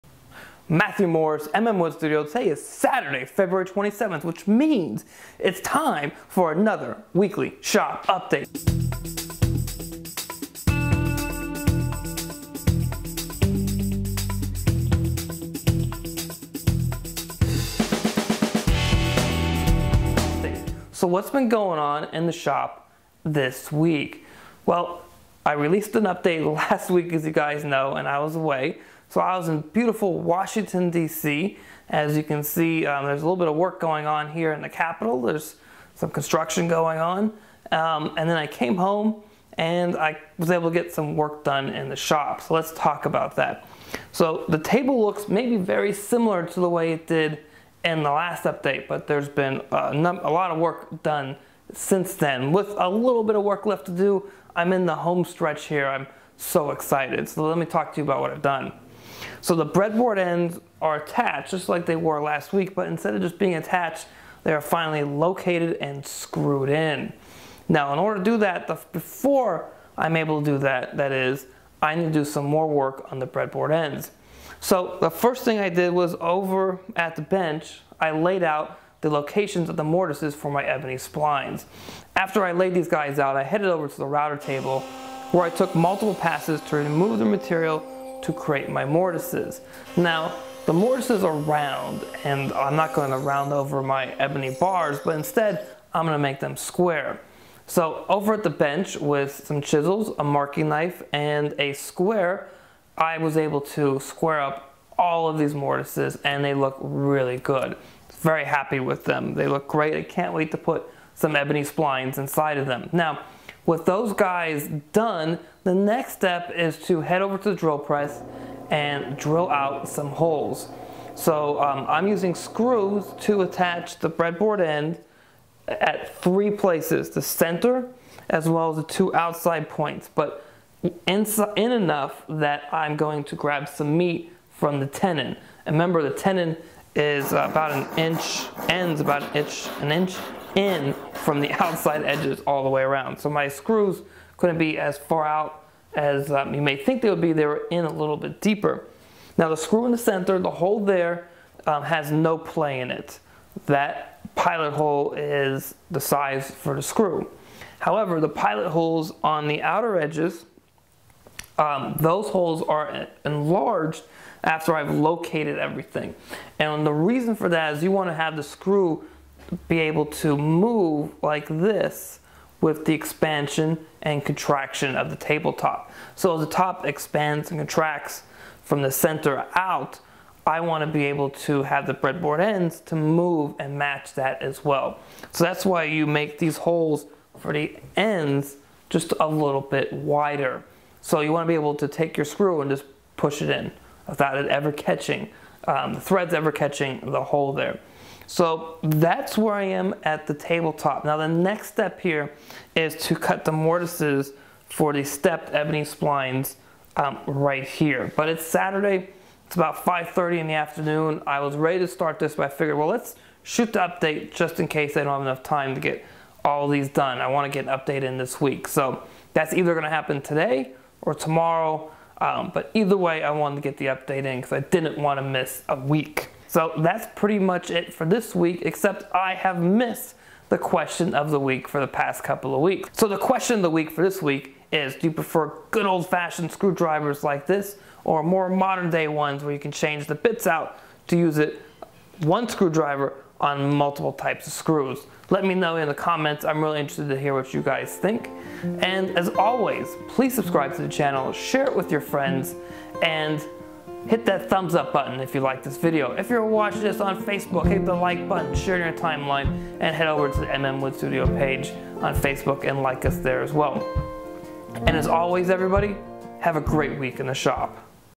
Weekly Shop Update February 27, 2016
This week I worked on the creating the ebony splines on the outside of the breadboard ends and the final fit and attachment of them to the table top.